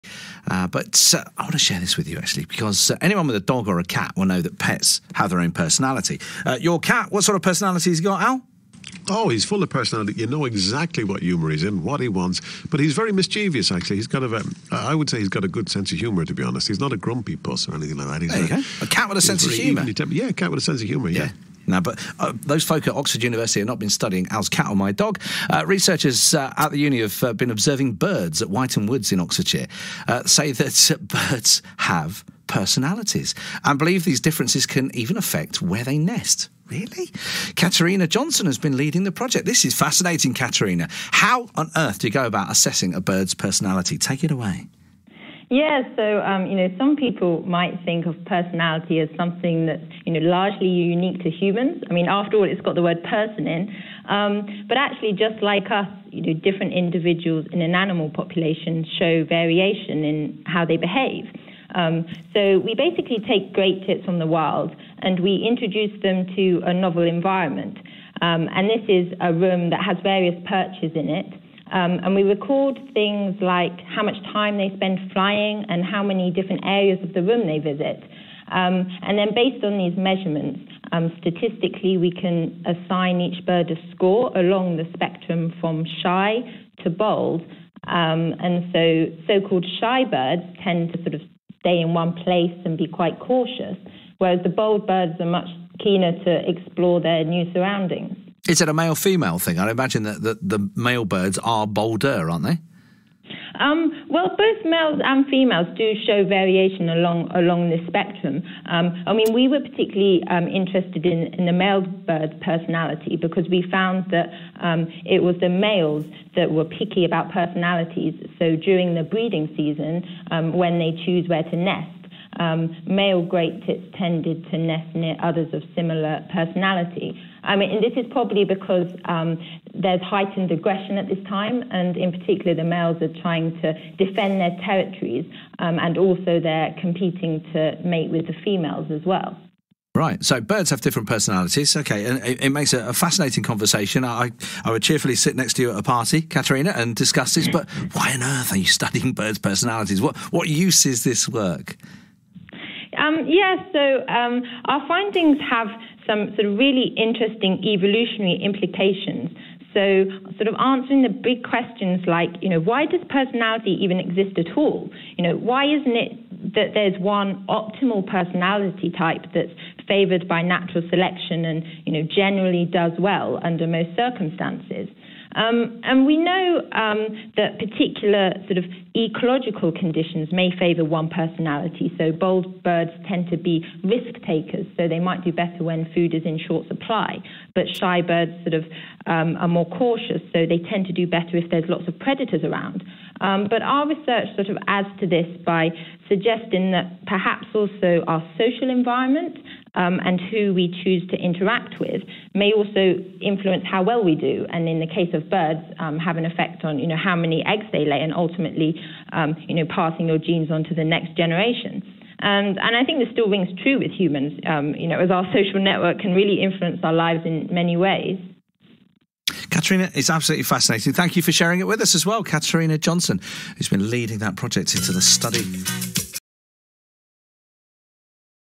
BBC Radio Oxford
Live radio interview May 2017 about my published research on the personality of great tits and their social networks.